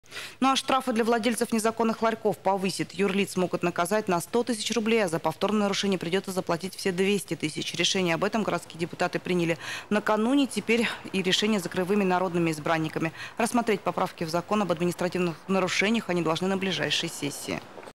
Штрафы для владельцев незаконных ларьков повысят
Юридических лиц могут наказать на 100 тысяч рублей, а за повторное нарушение придется заплатить все 200 тысяч